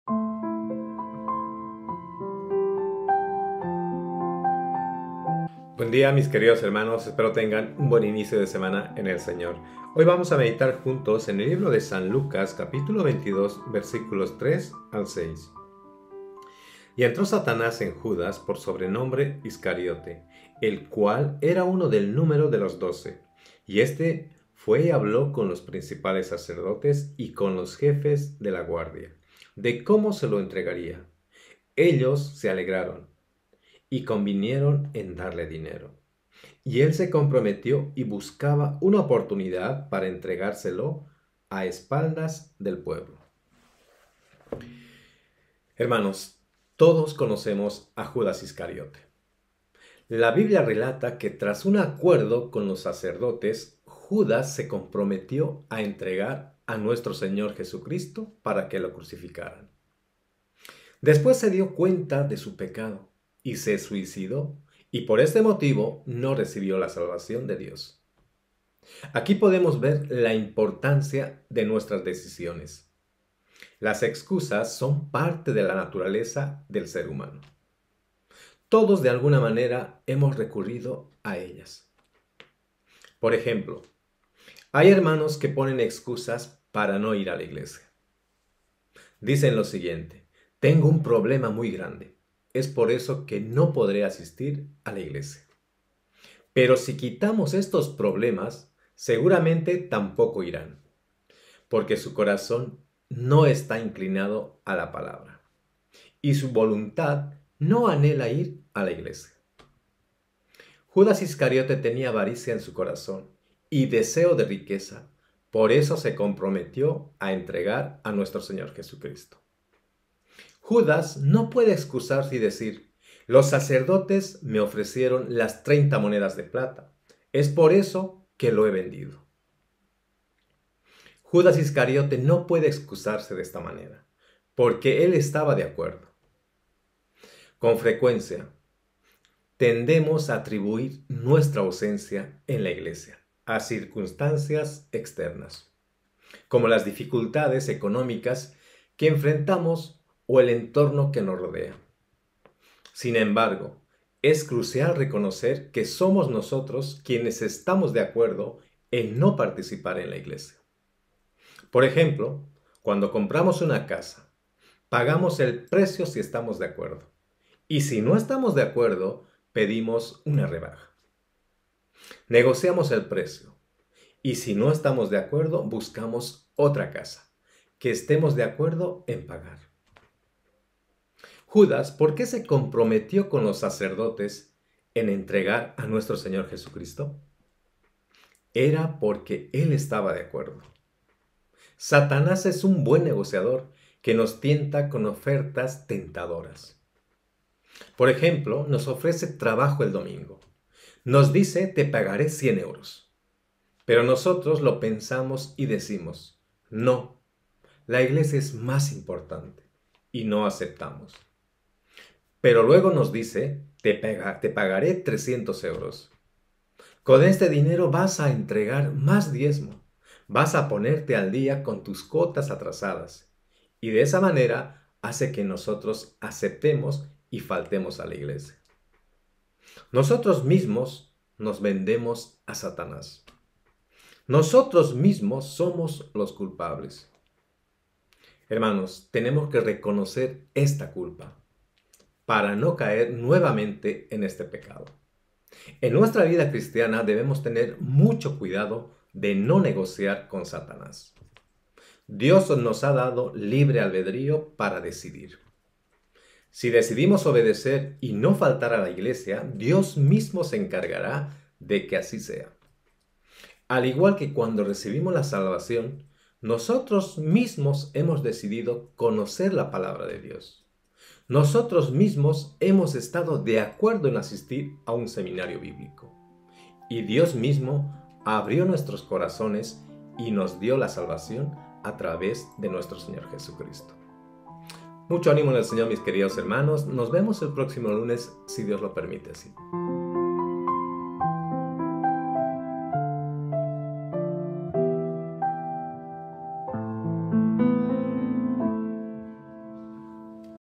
Meditación “No negocies con Satanás” - Lunes 22.01.2024
Evangelista Edgar
Título: No negocies con Satanás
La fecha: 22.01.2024